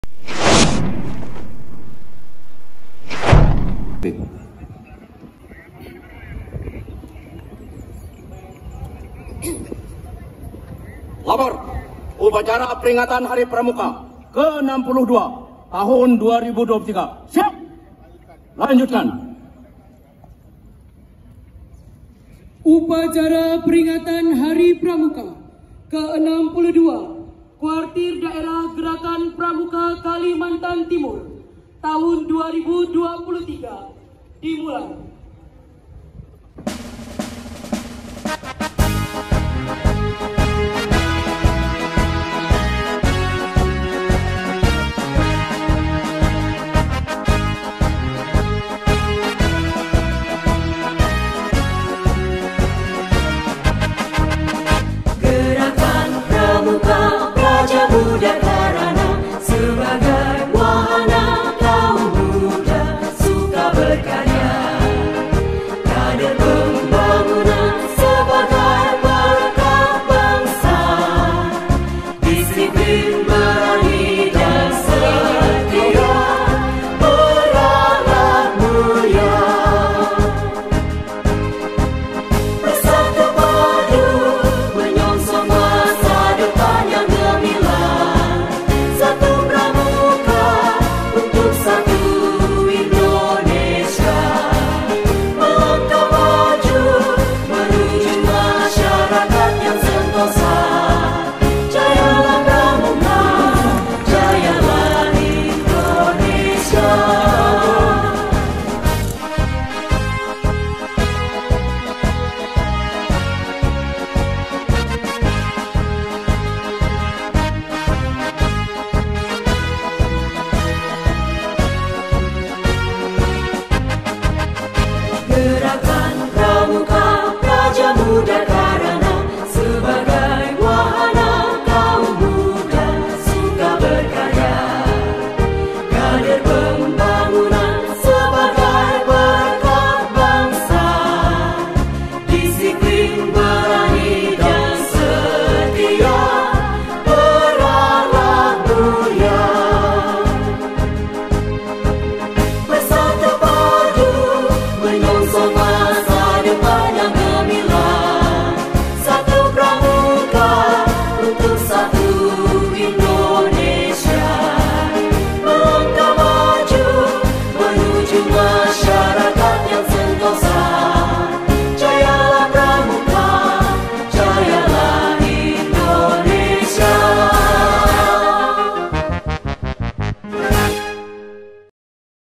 HUT Pramuka ke - 62 tahun 2023 kwardakaltim Di Balikpapan -Trailer
Memperingati Hari Pramuka ke - 62 tahun 2023 Kwartir Daerah Gerakan Pramuka Kalimantan Timur di Lapangan Upacara DOME Balikpapan,